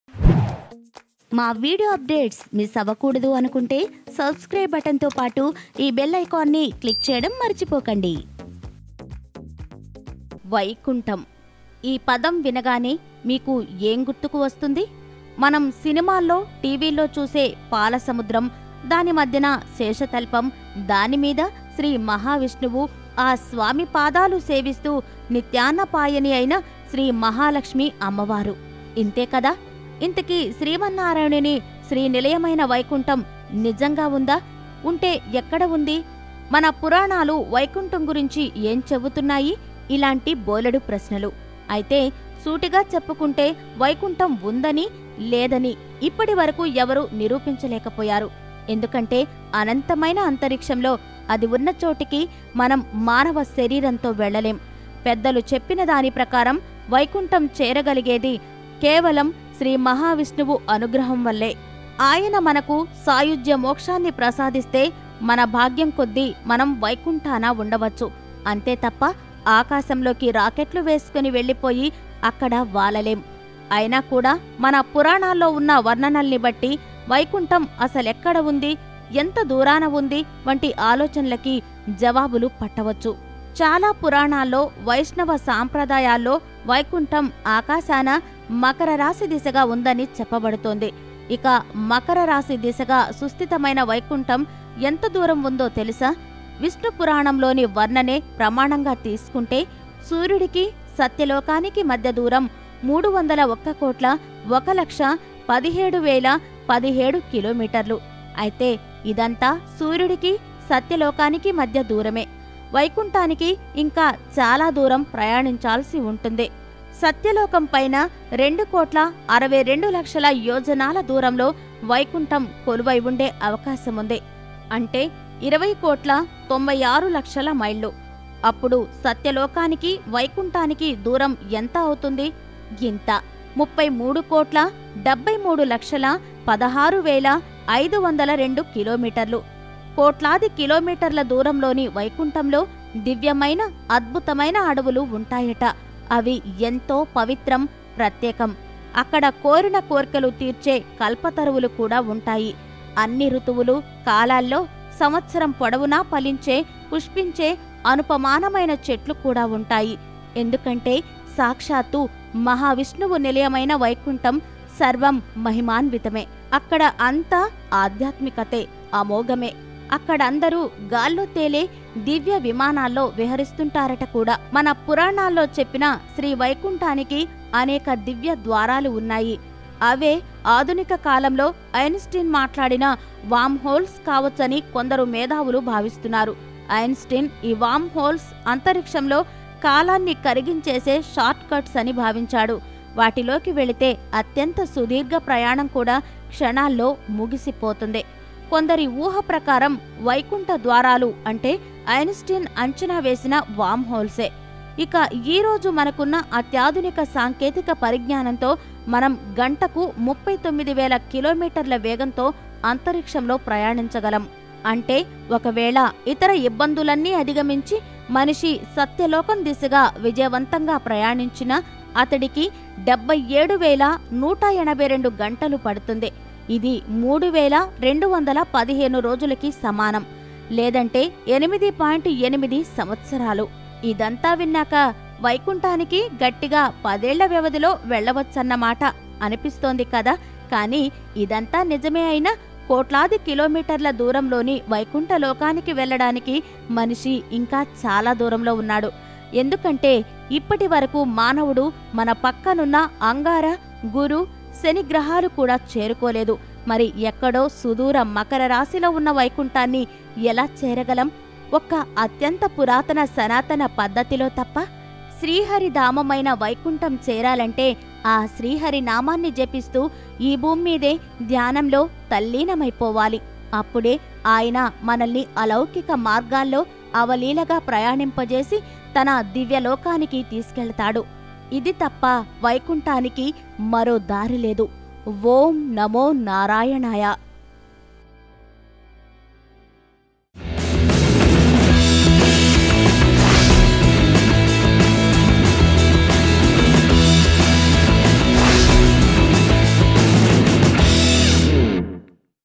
Unknown Facts About Lord Vishnu's Vaikuntham | వైకుంఠం గురించి ఆసక్తికరమైన నిజాలు | Telugu Panda
Unknown Facts About Lord Vishnu's Vaikuntham. Here Are The Complete Details About వైకుంఠం గురించి ఆసక్తికరమైన నిజాలు In This Video.